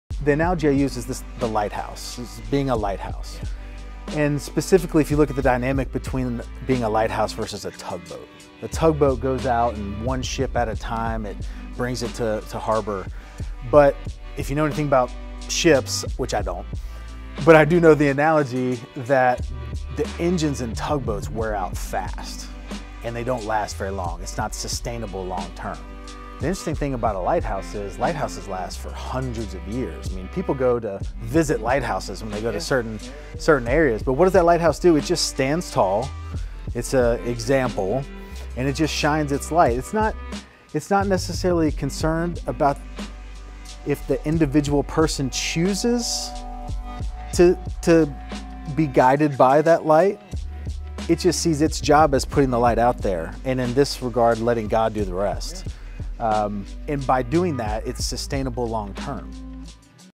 Be A Lighthouse | 🍞...Breadcrumbs with Tyler Harris
In this episode, Tyler talks about how to be a lighthouse in your own life, versus being a tugboat.